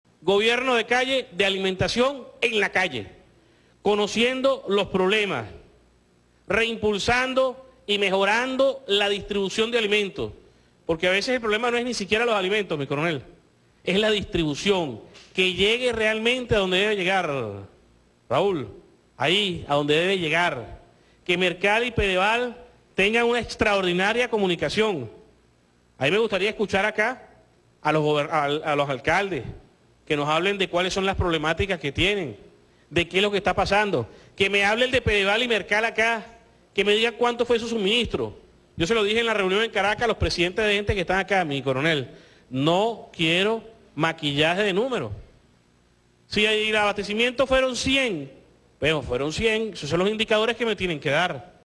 Rodolfo Marco Torres: No quiero maquillaje de cifras
ND / 10 ene 2016.- El nuevo ministro para la Alimentación, Rodolfo Marco Torres, pidió este domingo a sus subalternos que no le maquillen las cifras de distribución de alimentos a través de las redes públicas como Mercal y Pdval.

"Gobierno de alimentación en la calle, conociendo los problemas, reimpulsando y mejorando la distribución de alimentos porque a veces, mi coronel - en referencia al gobernador de Apure, Ramón Carrizales, presente en la reunión- es la distribución. Que llegue realmente a donde debe llegar... que Mercal y Pdval tengan una extraordinaria comunicación... que me hablen acá de Mercal y Pdval de cuánto fue su suministro. Yo se los dije en Caracas a los presidentes de entes que están acá, mi coronel; no quiero maquillaje de números. Si el abastecimiento fueron 100, bueno, fueron 100. Esos son los indicadores que me tienen que dar".